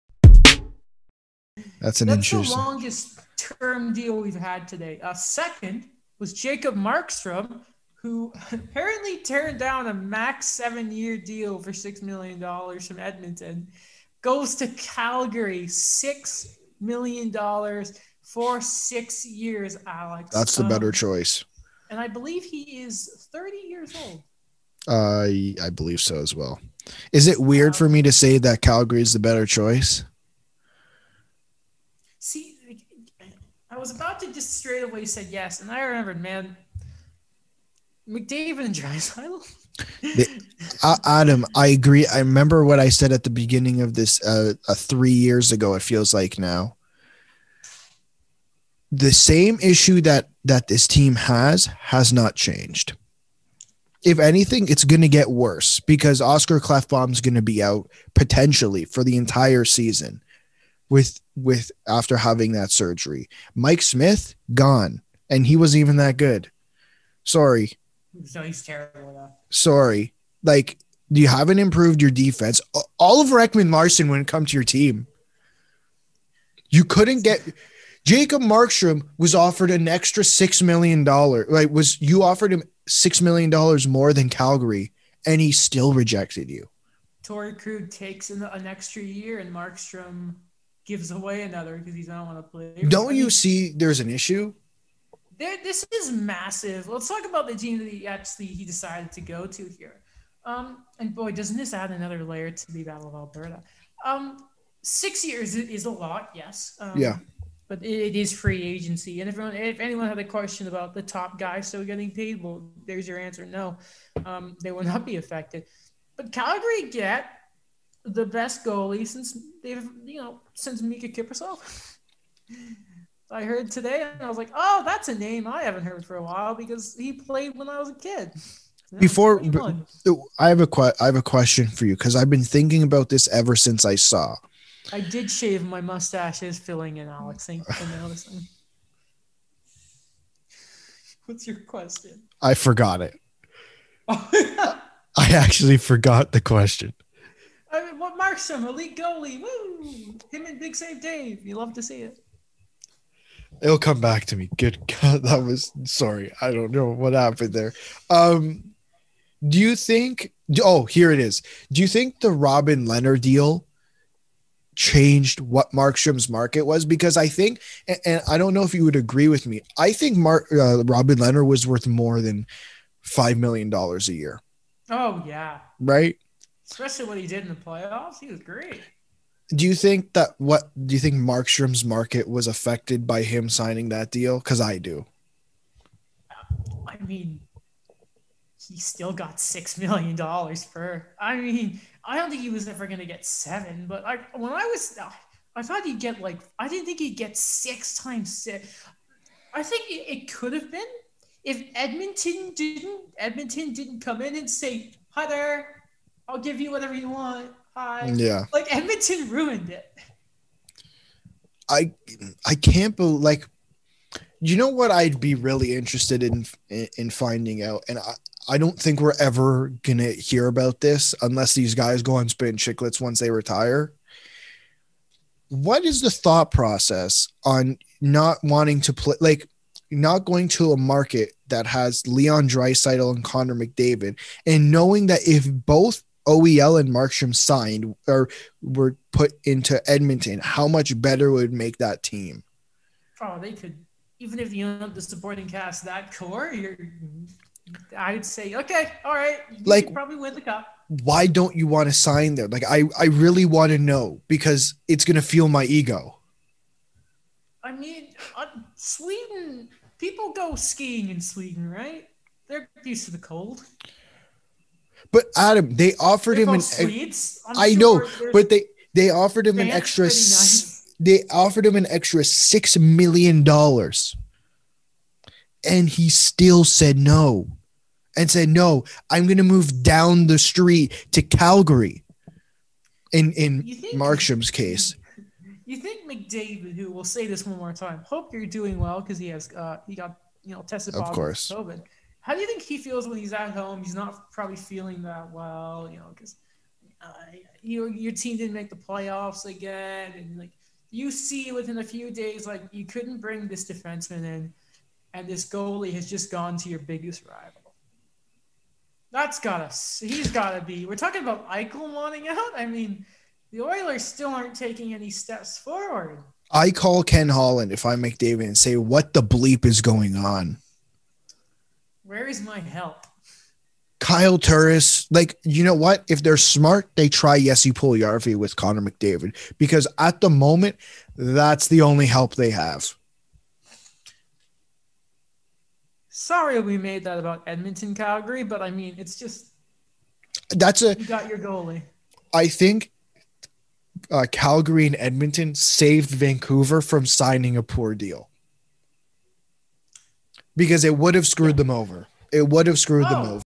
Why Can't Edmonton Do Anything
In this clip, we talk about the Edmonton Oilers lack of action in the goalie market at the start of Free Agency